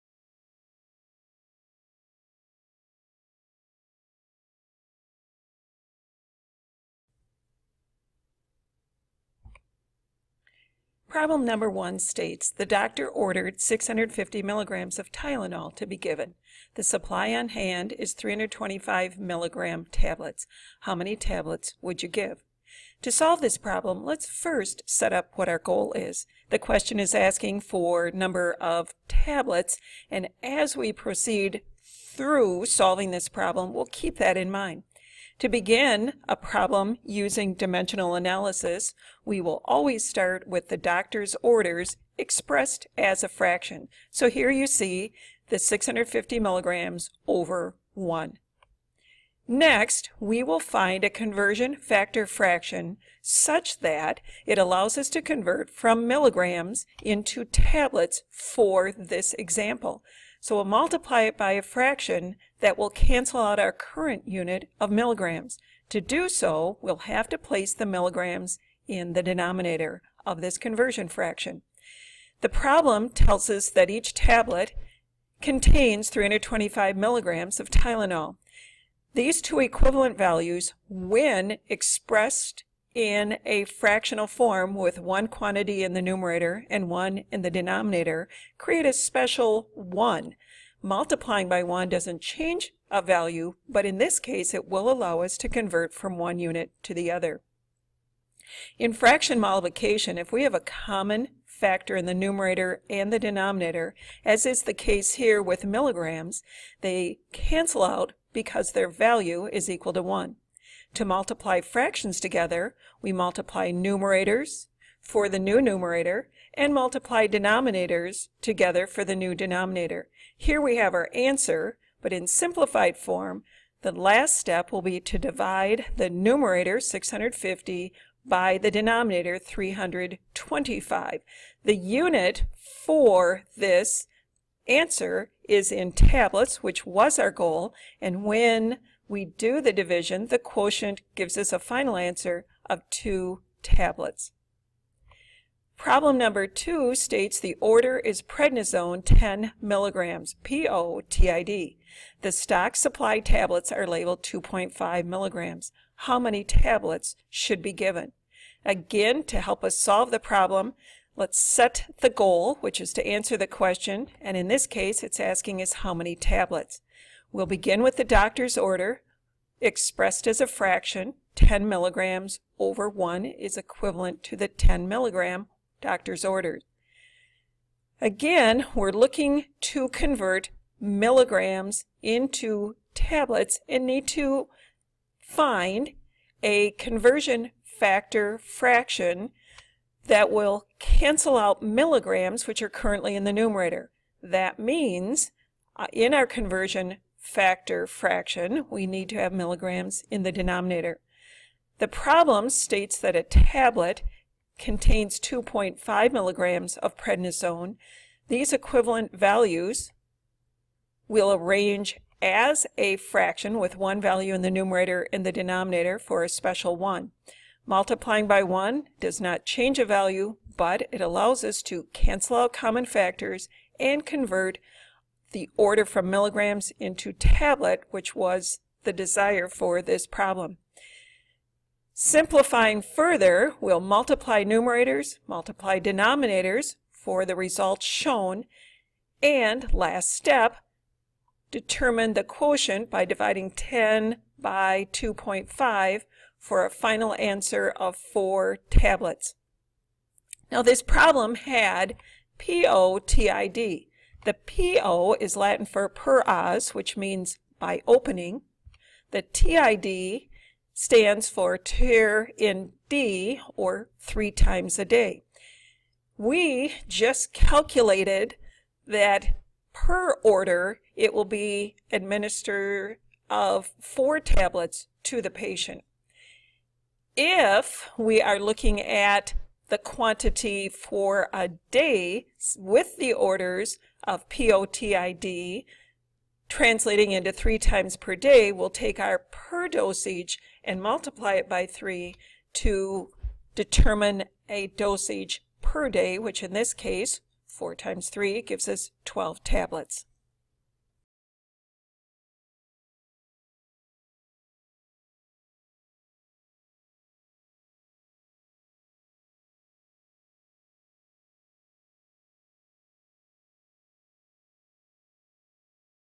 Entry-Level Drug Calculations for Nursing Students Part 1 – Pharmacology, Nursing Math*
Learn how to perform entry-level drug calculations using dimensional analysis.  Video 1 of 7.

Problems 1.) and 2.) of the worksheet are demonstrated;  convert doctor’s order (in milligrams) to quantity of tablets to give the patient.

For a comprehensive and affordable ($5.00) eBook, check out "Dimensional Analysis in Nursing, 2nd Edition" by Pete Esser;

This video was created and narrated by Helen Mar Adams.  Southwest Wisconsin Technical College math and science instructor.